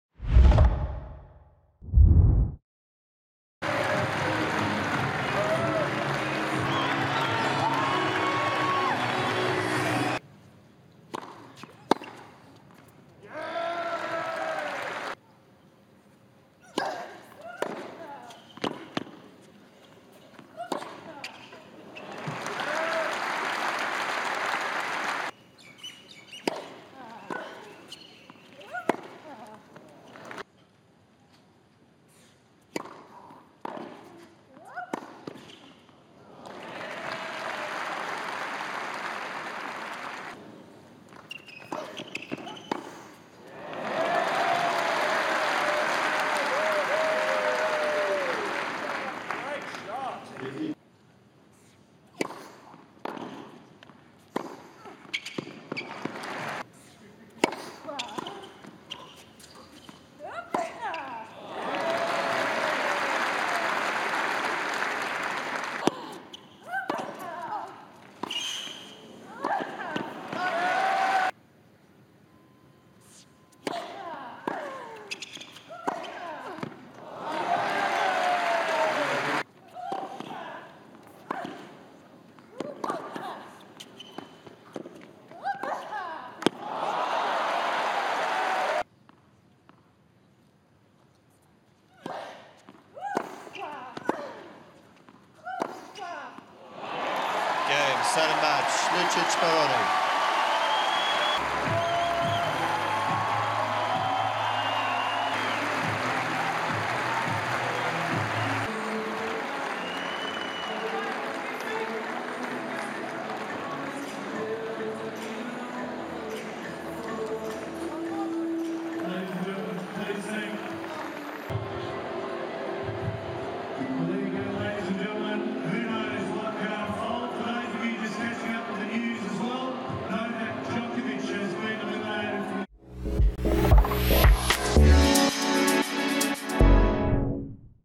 Lucic-Baroni v Radwanska match highlights (2R) | Australian Open 2017
Match highlights from the second round of singles at the Australian Open 2017.